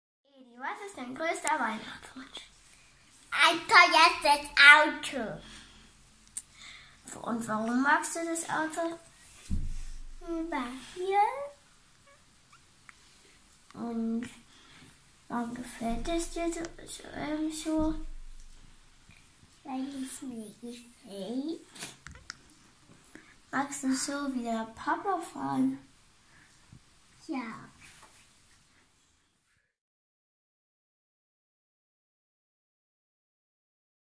dietoreweit - Weihnachtswunsch - 2
„Was ist Dein größter Weihnachtswunsch?“,
das hat unser Weihnachts-Reporter Martin auch seinen kleinen Bruder gefragt. Und Elis Wunsch ist ganz schön groß!